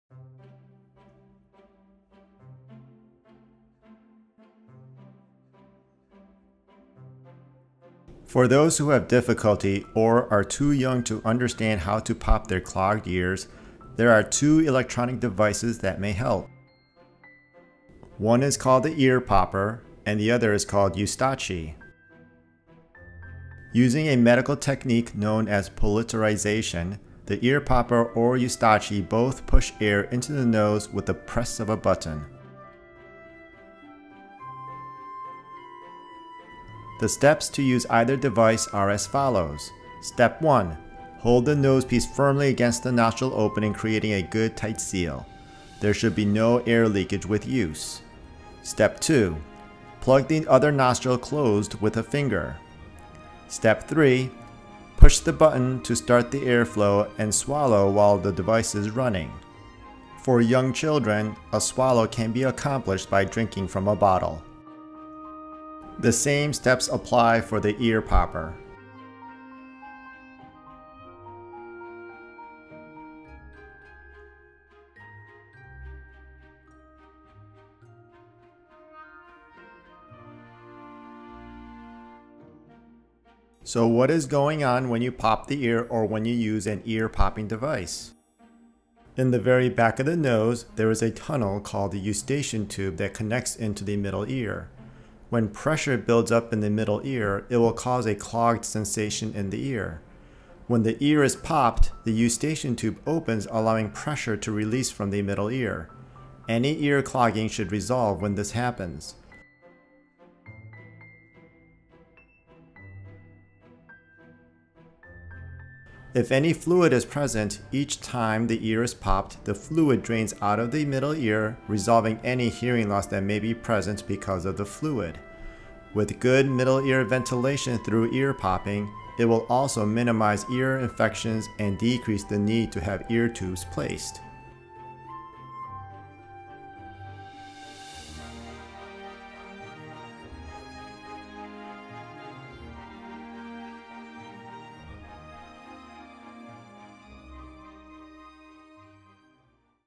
EarPopper or Eustachi Therapy to Treat Clogged Ears, Fluid in Ears, and Ear Infections
This video explains how to use the EarPopper or Eustachi (0:39) to help resolve clogged ears, remove fluid from the ears, and minimize ear infections in some adults and especially in young children who do not yet know how to pop the ears the normal way. This video shows a 3 years old using the EarPopper and Eustachi. Anatomic explanation is also provided in the second half of the video (1:22).

There is a nasal balloon called Otovent which also can be tried.